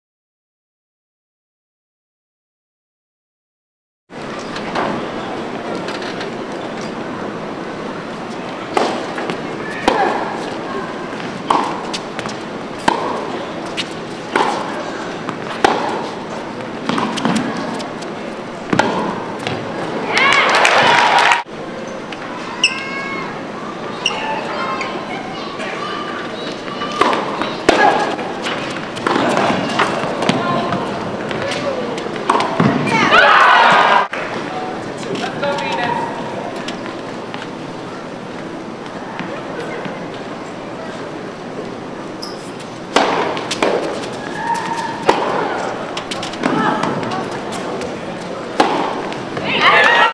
Venus Williams in a Woman's Singles Match : Philadelphia Freedoms vs Washington Kastles
Venus Williams of the Washington Kastles defeats Noppawan Lertcheewakarn of the Philadelphia Freedoms in a World TeamTennis match on July 8th in Philadelphia.